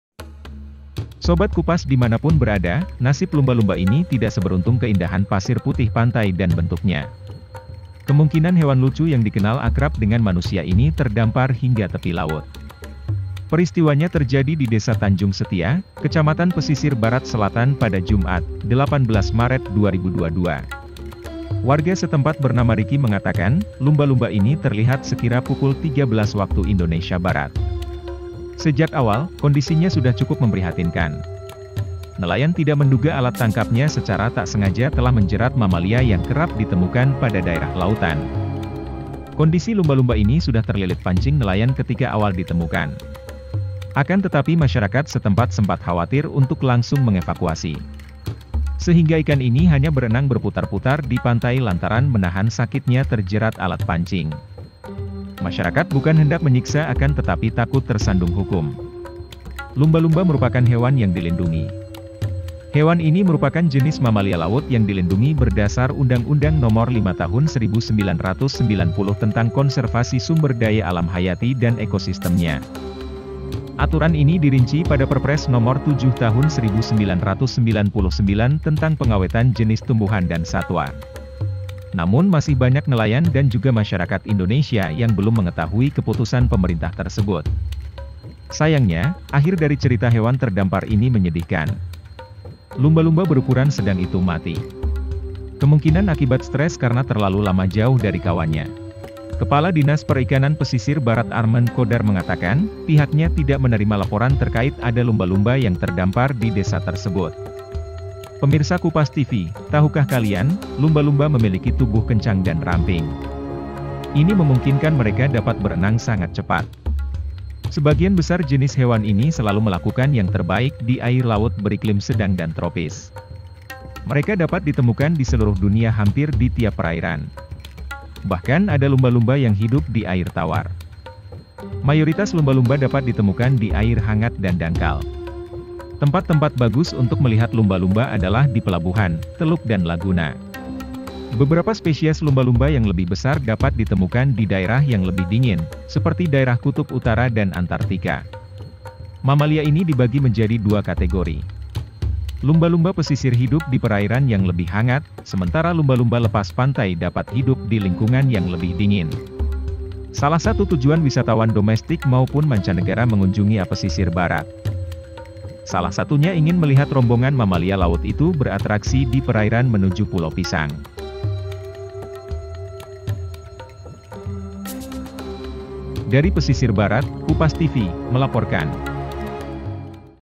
NASIB LUMBA-LUMBA DI TANJUNG SETIA PESISIR BARAT YANG TERLILIT PANCING NELAYAN
Nasib lumba-luma di Tanjung Setia Pesisir Barat Bernasib Naas karena terlilit pancing nelayan di Tanjung Setia Pesisir Barat sehingga mengakibatkan lumba-lumba terdampar di tepi Pantai Tanjung Setia Pesisir Barat

Pesisir Barat, Nasib lumba-lumba ini tidak seberuntung keindahan pasir putih pantai dan bentuknya. Kemungkinan hewan lucu yang dikenal akrab dengan manusia ini terdampar hingga tepi laut. Peristiwanya terjadi di desa Tanjung Setia, Kecamatan Pesisir Barat Selatan pada Jumat, 18 Maret 2022.

Warga setempat bernama Riki mengatakan, lumba-lumba ini terlihat sekira pukul 13.00 WIB. Sejak awal, kondisinya sudah cukup memprihatinkan. Nelayan tidak menduga alat tangkapnya secara tak sengaja telah menjerat mamalia yang kerap ditemukan pada daerah lautan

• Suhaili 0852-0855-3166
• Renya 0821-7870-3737